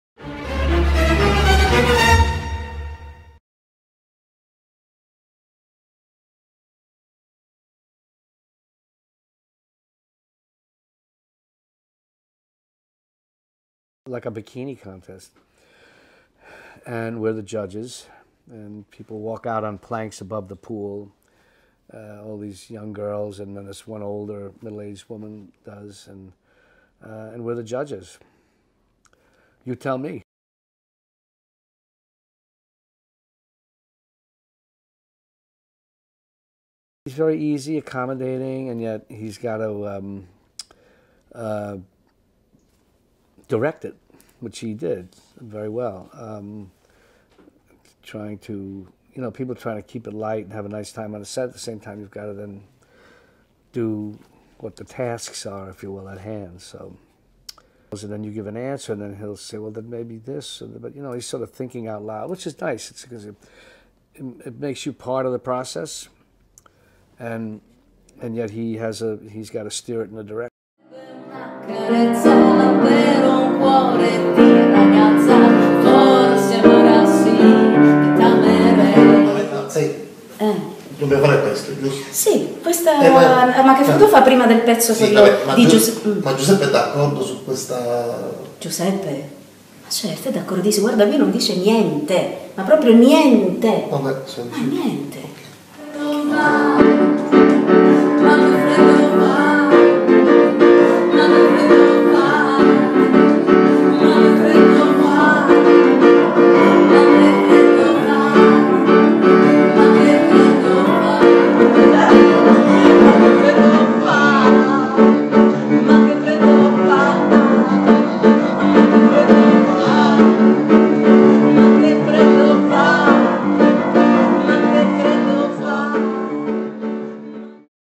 Moschella&Mulè con Robert De Niro mp4
Un evento più unico che raro: il duo  Moschella&Mulè con Robert Deniro, al piano Diego Spitaleri. Regia Giuseppe Moschella, aiuto regia Mirna Summa.